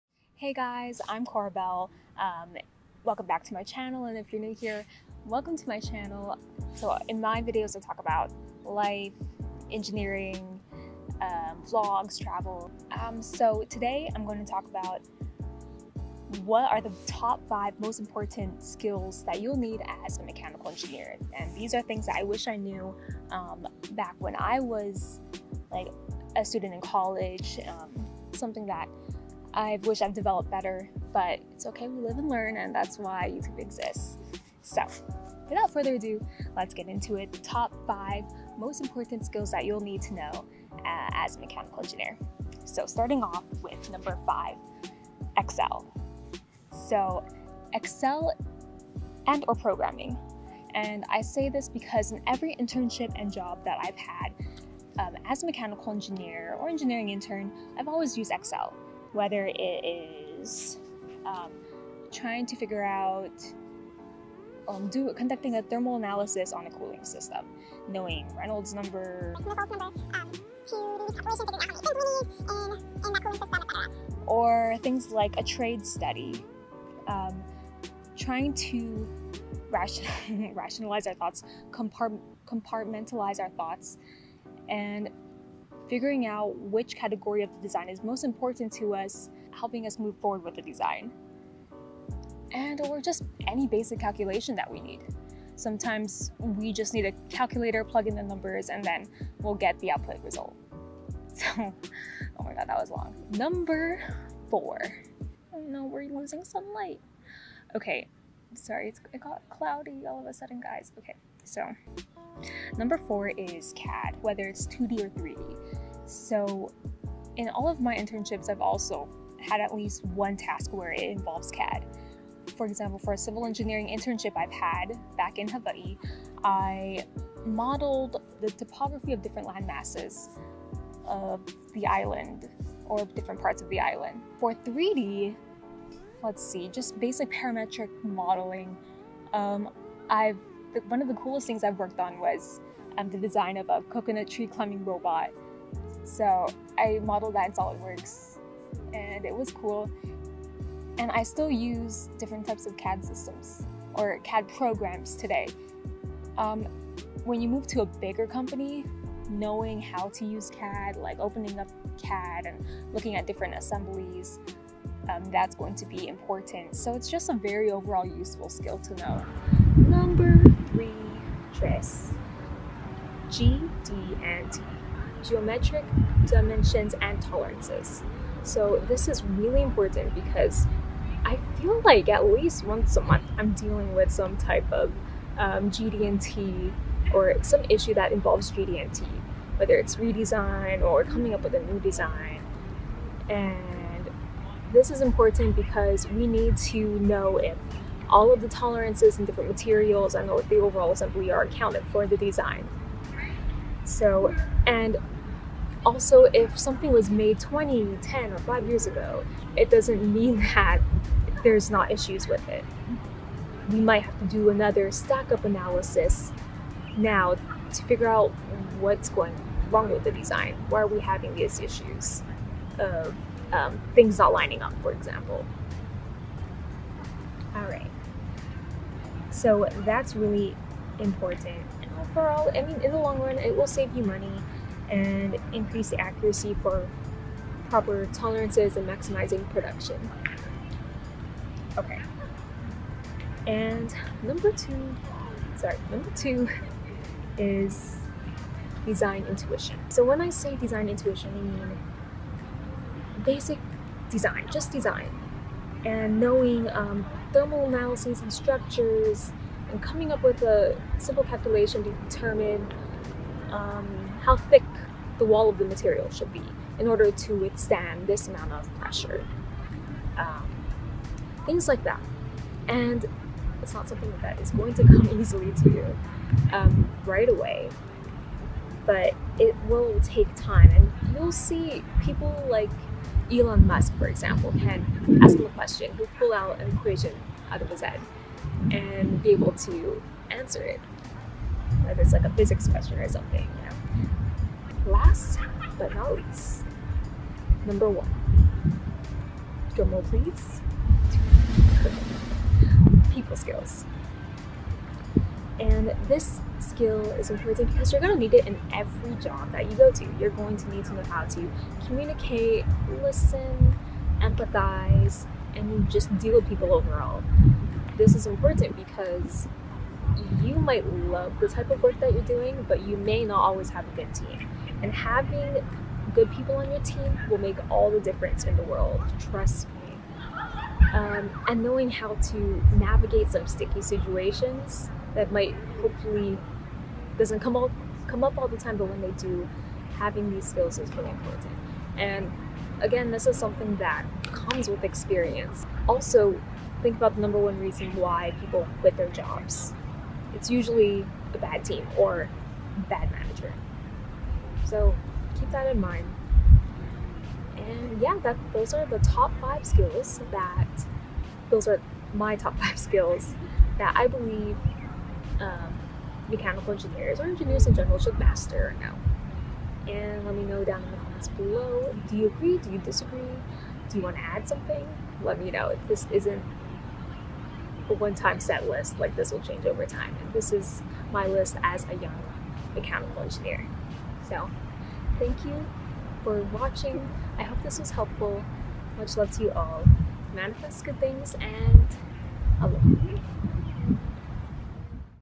TOP 5 SKILLS You need as a Mechanical Engineer 2022
Learn the top 5 skills you need as a mechanical engineer!

Top 5 Most Important Skills:
5. Excel/Programming
4. CAD
3. GD&T - Geometric Dimensioning and Tolerancing
2. Design Intuition
1. People Skills

The engineering world is competitive, so if you know or master any of these skills, this will put you ahead of the rest of the crowd (or help you be the best engineer you can be). Everyone's experience in engineering is different, but the core principles are the same.